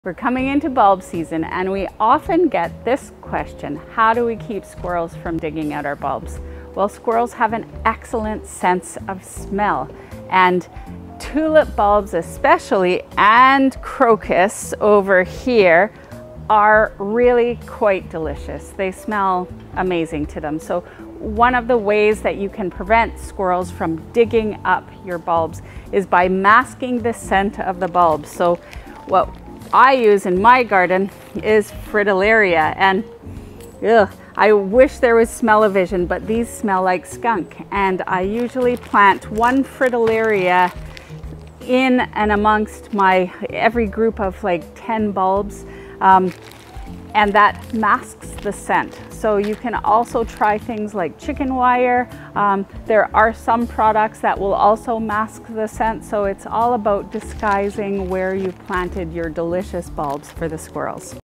How do we keep squirrels from digging up our fall bulbs?
We Get This Question All the Time
We get this question a lot during bulb season: How do we keep squirrels from digging up our fall bulbs?

Well, squirrels have an excellent sense of smell, and tulip bulbs, especially, and crocus are really quite delicious and they smell amazing to them. One of the ways that you can prevent squirrels from digging up your bulbs is by masking the scent of the bulbs. So what I use in my garden is Fritillaria, these smell like skunk! And I usually plant one Fritillaria in and amongst my every group of like 10 bulbs. And that masks the scent! You can also try things like chicken wire or try products that will also mask the scent.

So it's all about disguising where you planted your delicious bulbs for the squirrels.